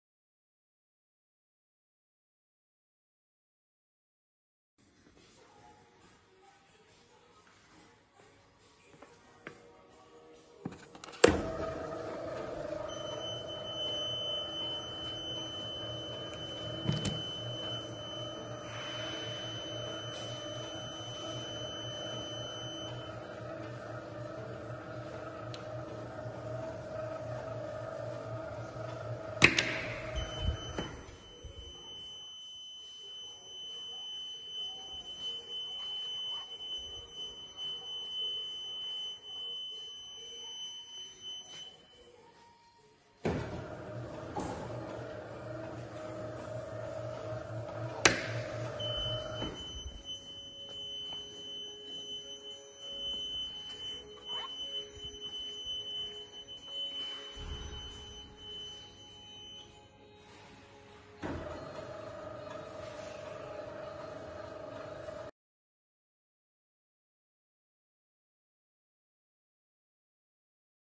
645-3: Ceia metal detector
Ceia metal detector, type: THS/3F, serial number: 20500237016, plastic belt, belt width: 250 mm, clearance: 300 mm x 300 mm, total dimensions: 2000 mm x 950 mm x 1150 mm.